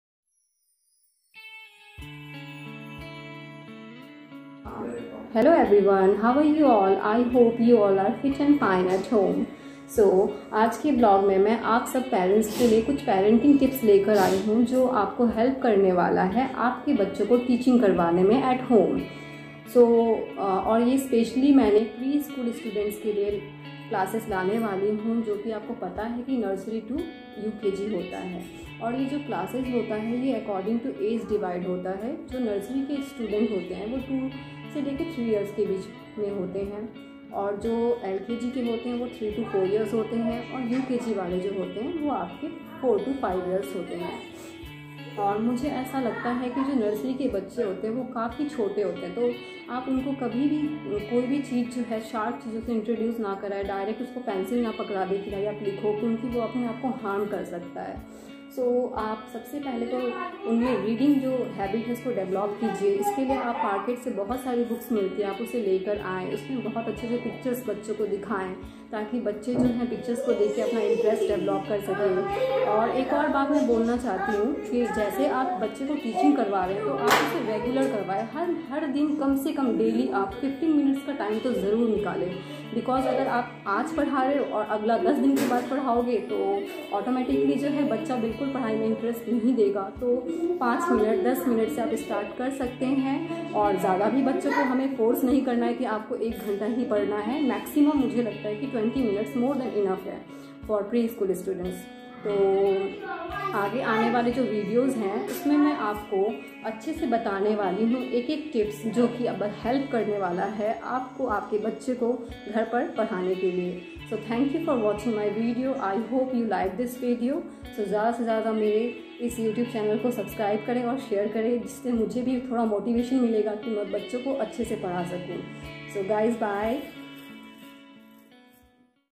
PRE SCHOOL PARENTING TIPS
HELLO FRIEND THIS VIDEO I HAD MADE ESPECIALLY FOR PARENTS WHO HAVE SMALL AGE CHILDREN TO HELP THEM IN TEACHING THEIR KIDS AT HOME